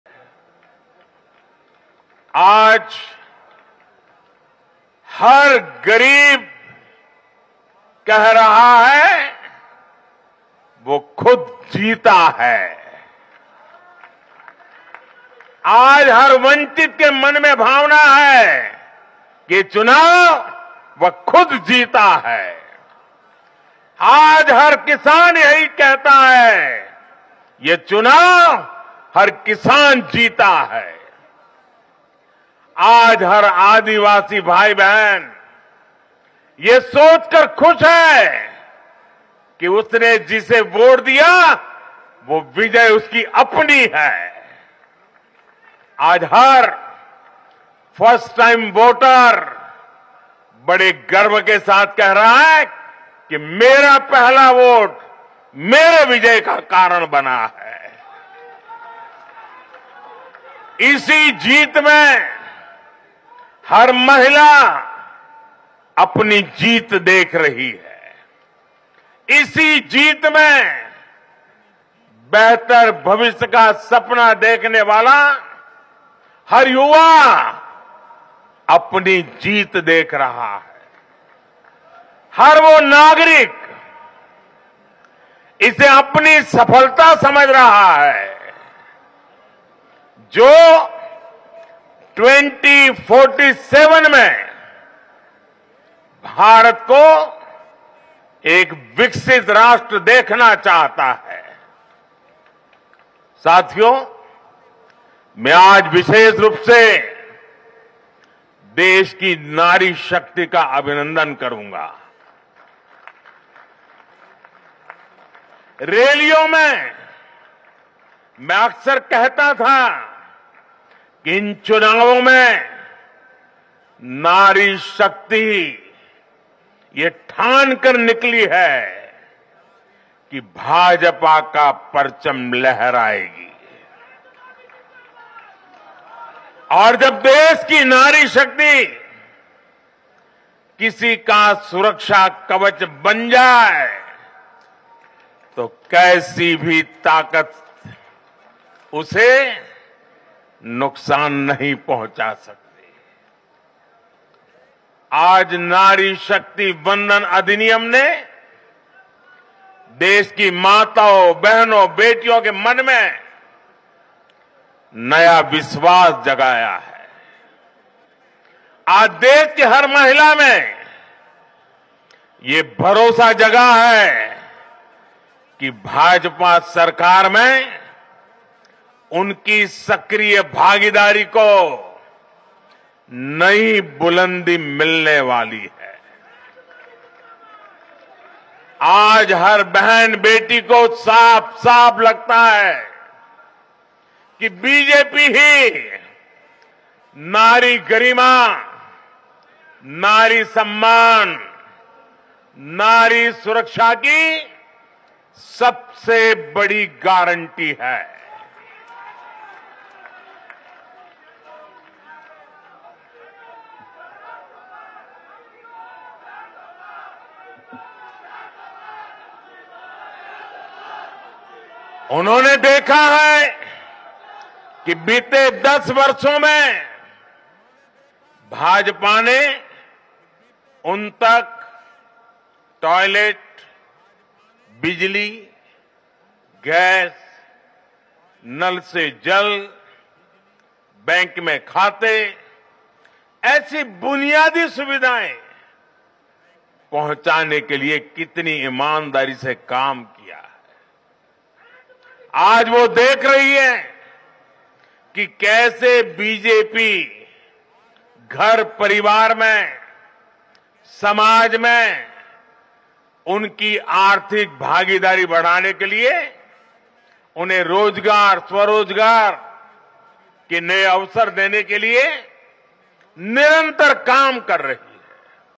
Narendra Modi Speech After Winning 3 States in Assembly Election | भाजपच महिला सन्मानाची गॅरंटी आहे
Vidhan Sabha Nivdnuk 2023 : Narendra Modi Speech After Winning  3 States in Assembly Election | भाजपच महिला सन्मानाची गॅरंटी आहे

Uploaded by : MNK